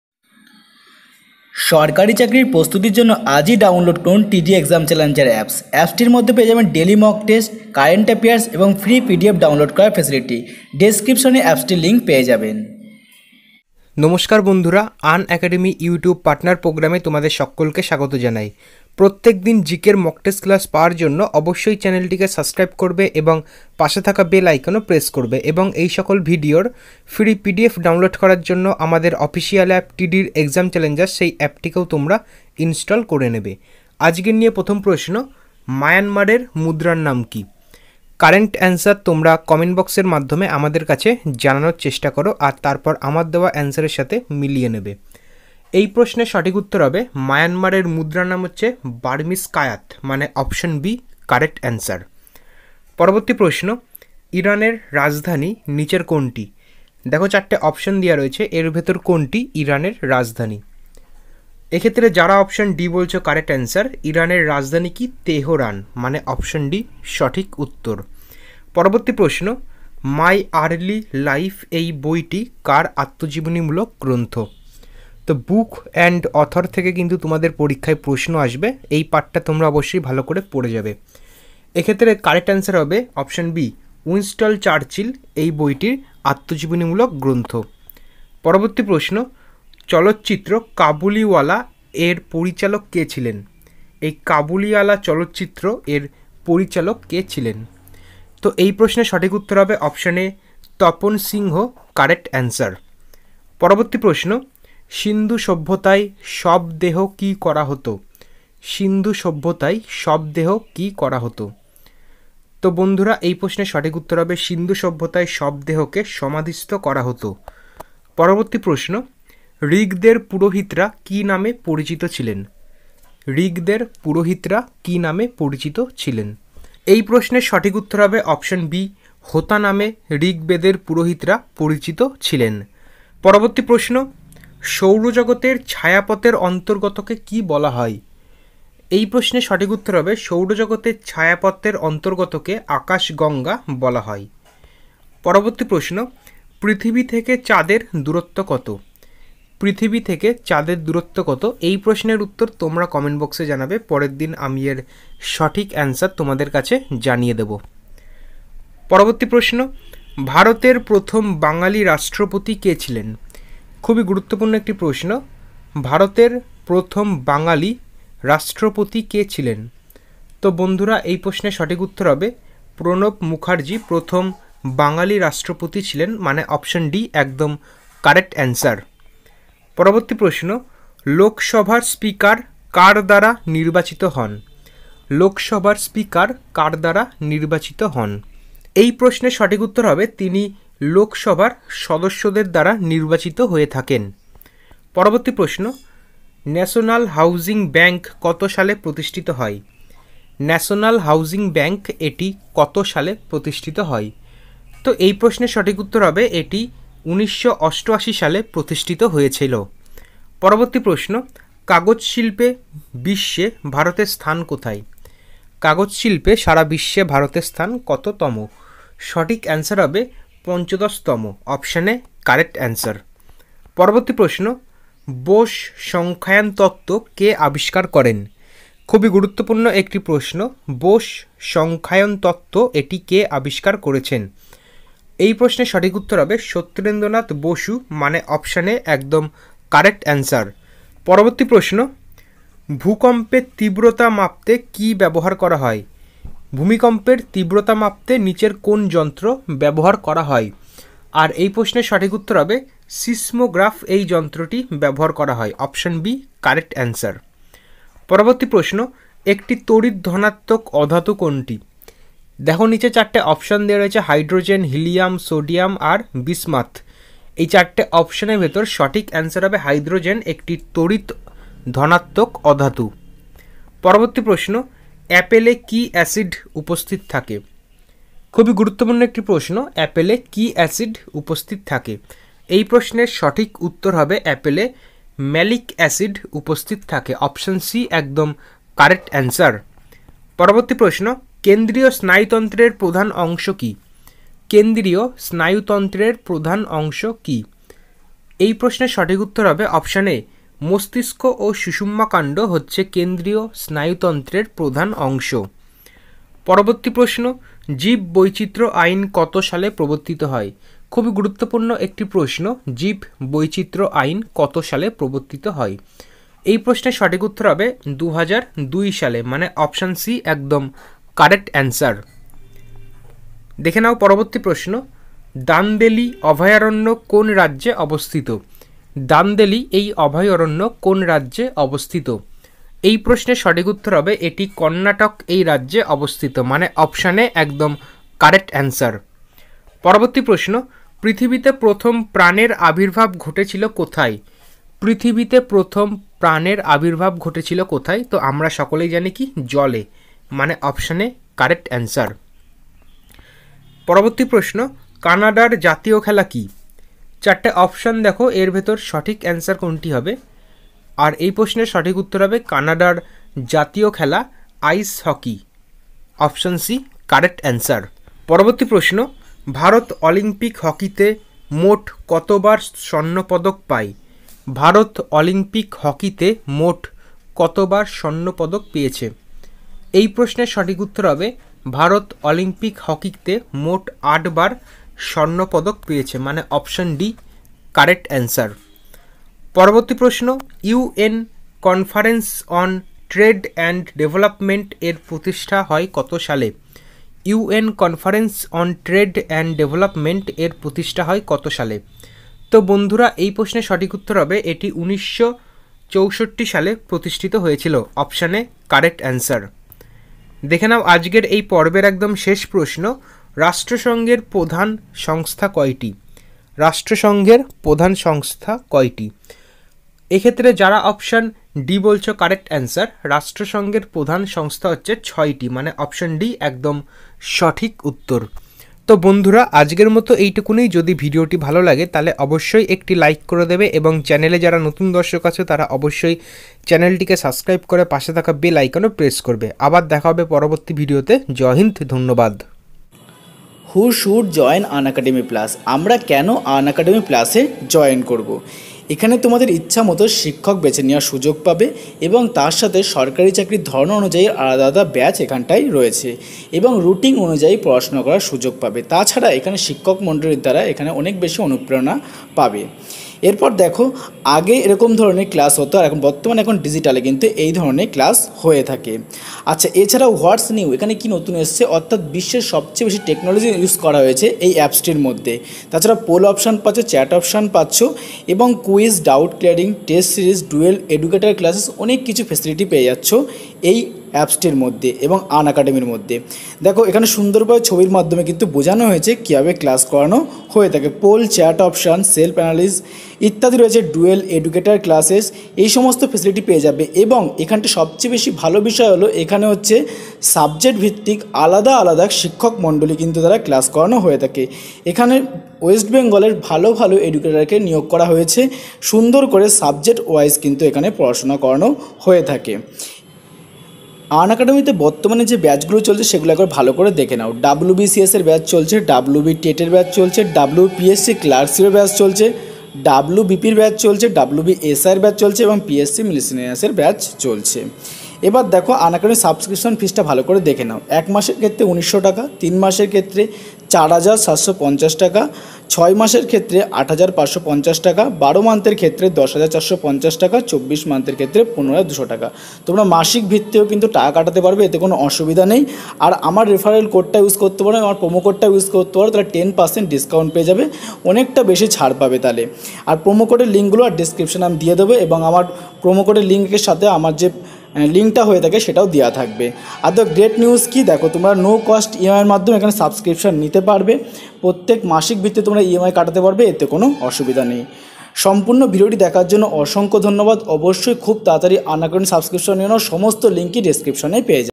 WB Police important General knowledge class | Target Sarkari job
General knowledge mock test class for any competitive exam . excise main kolkata police west bengal police rail for any government job important G.k class .

Daily mock test in bengali for excise main rail ssc psc wb police 2020